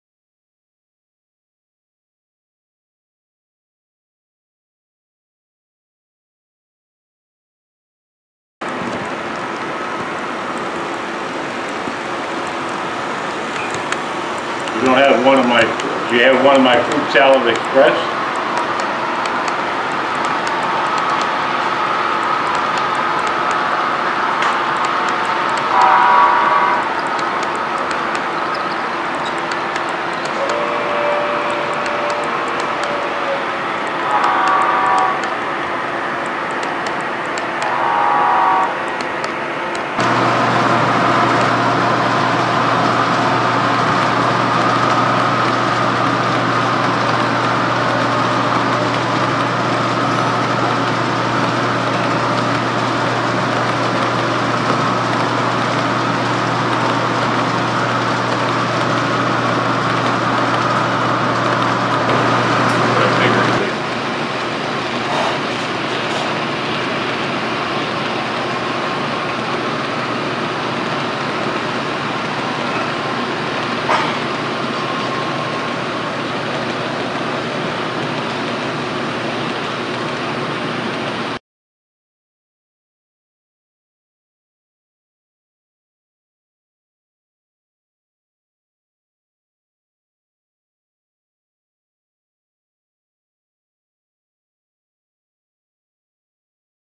Santa Fe War Bonnet Pt. 3 @ Lake Shore Model Railroad Association
The ever popular Santa Fe War Bonnet scheme will always grace the rails at Lake Shore Model Railroad Association.

Memberships are available.